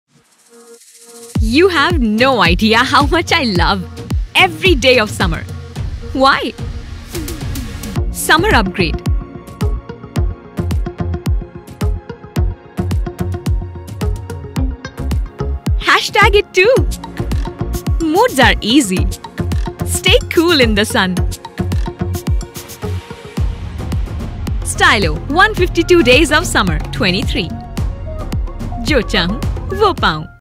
Stylo Summer Upgrade'23
Done with our upgrade! Now you can see and get your feet into those lovely, lively, pretty pairs of shoes from Stylo Summer Upgrade ’23! Get into those sunny vibes, feel good, wear colorful because it’s now time to upgrade your shoe closet too!